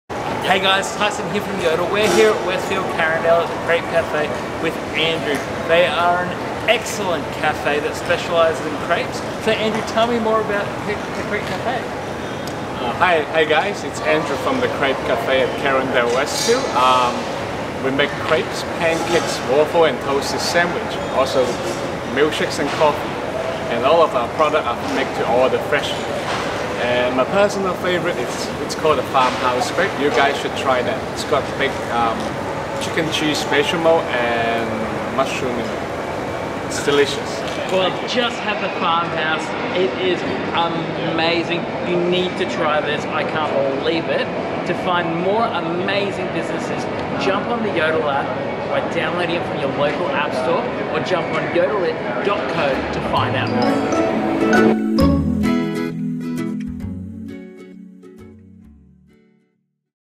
Crepe Cafe & Yodel Partnership
The Crepe Cafe Carindale has been one of our standout locations we have visited in the last month! We are in love with their farmhouse crepe!